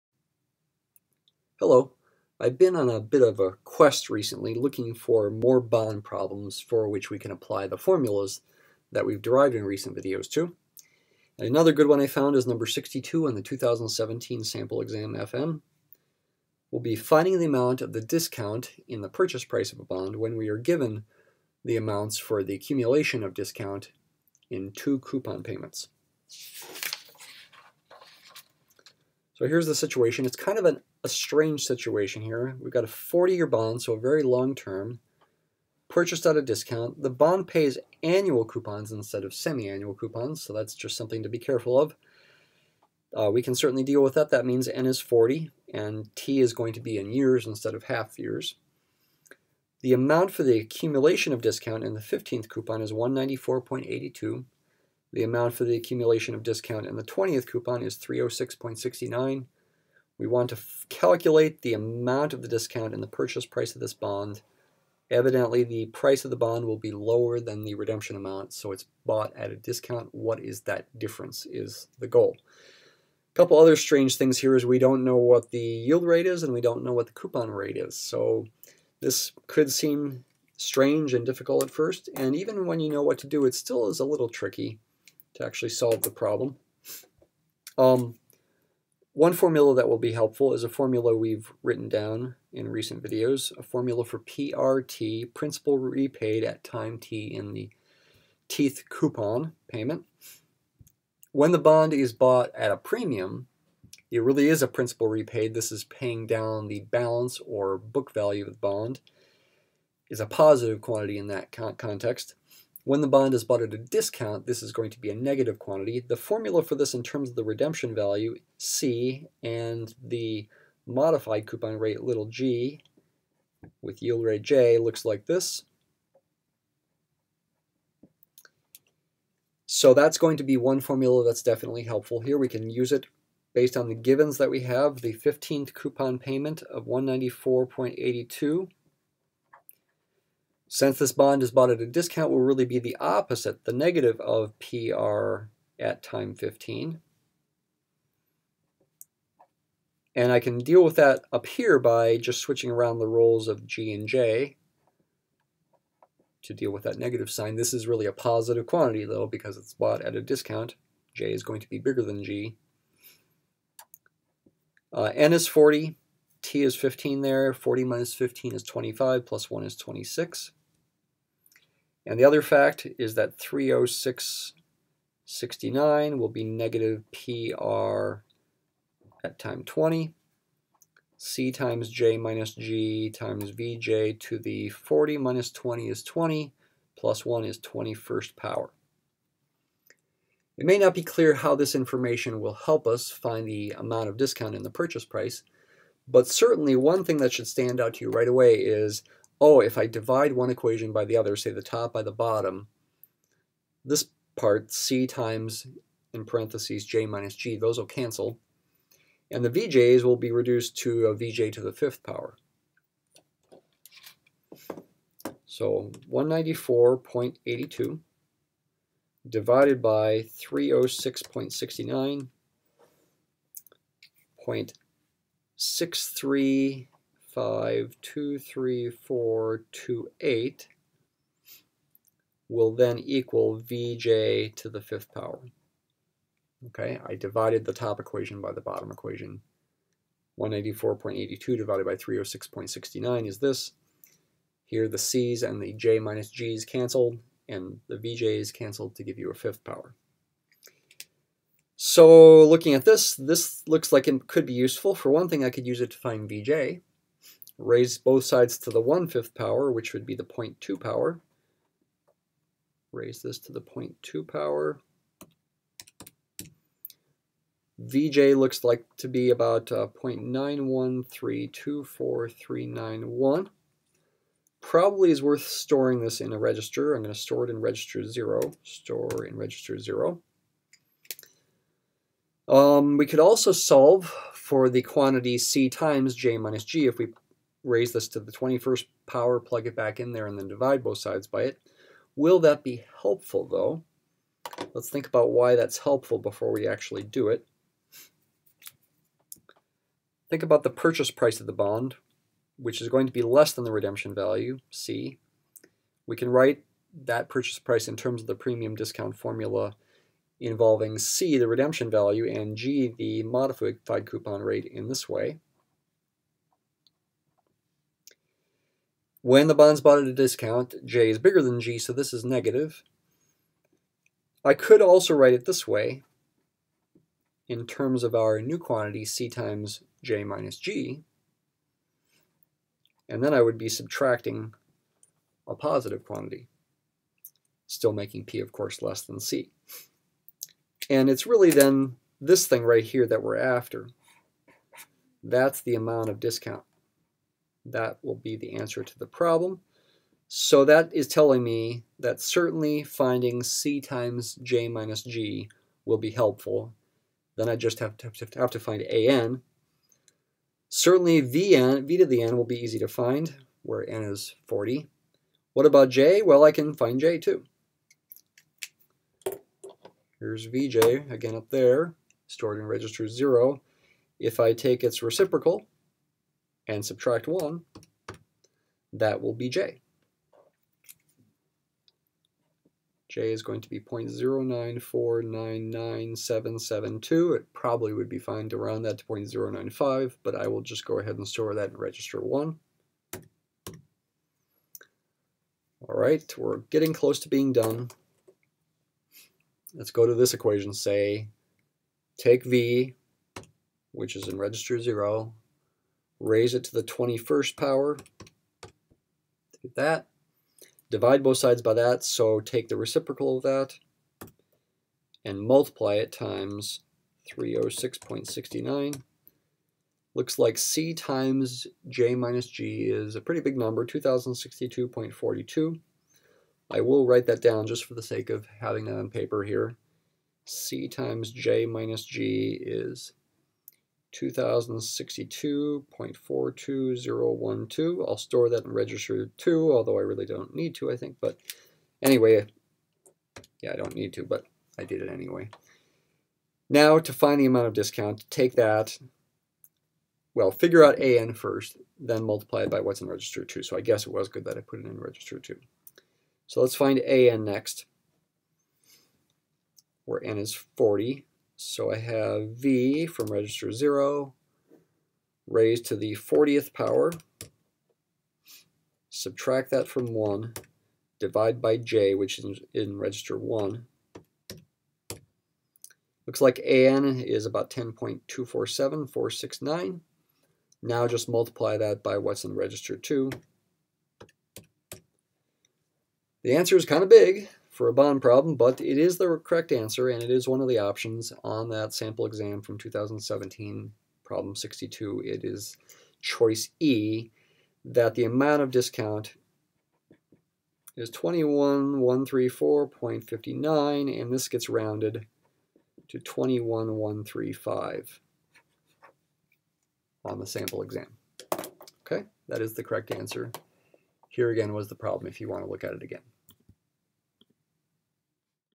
Actuarial Exam 2/FM Prep: Amount of Discount on a Bond Given Accumulation of Discount for 2 Coupons
Financial Math for Actuarial Exam 2 (FM), Video #113.  Sample 2017 Exam FM Problem #62.

Sample 2017 Exam FM Problem #62: A 40-year bond is purchased at a discount. The amount for the accumulation of discount in the 15th coupon is 194.82. The amount for accumulation of discount in the 20th coupon is 306.69. Calculate the amount of discount in the purchase price of this bond.